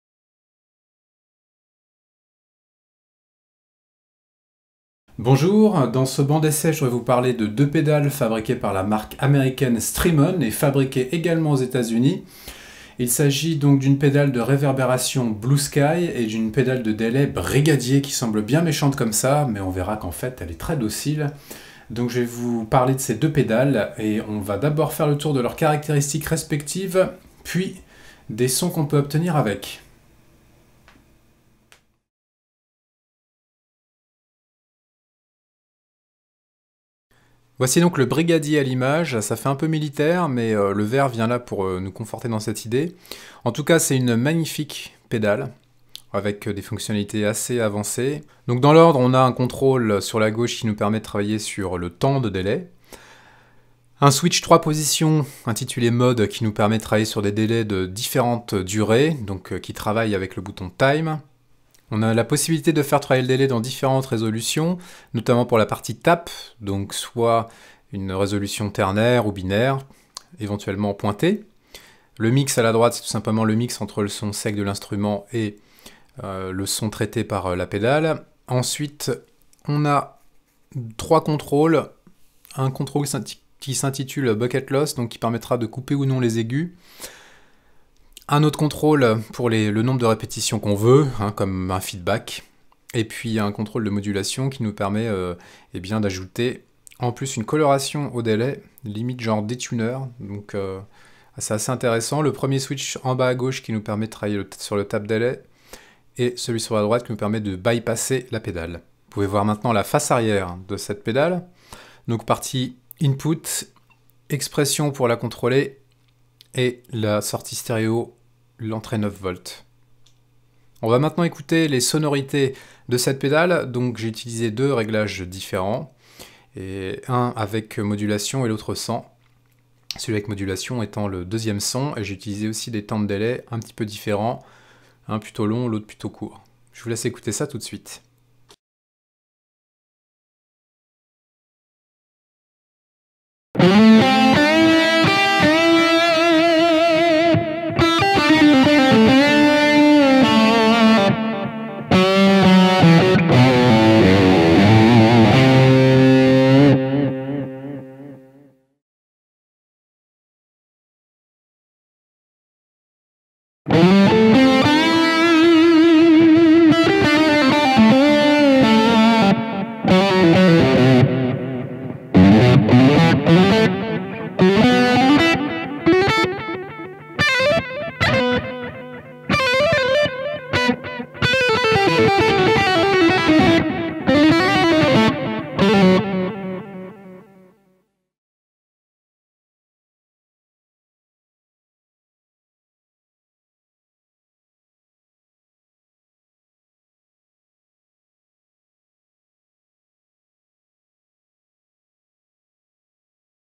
[TEST] strymon brigadier
Strymon est une marque de pédales entièrement fabriquée au USA. Et en attendant le test de la très convoitée El Capistan, revenons sur le delay Brigadier, une pédale très complète et puissante. Difficile, même pour le prix, de trouver des challengers pouvant s'y mesurer.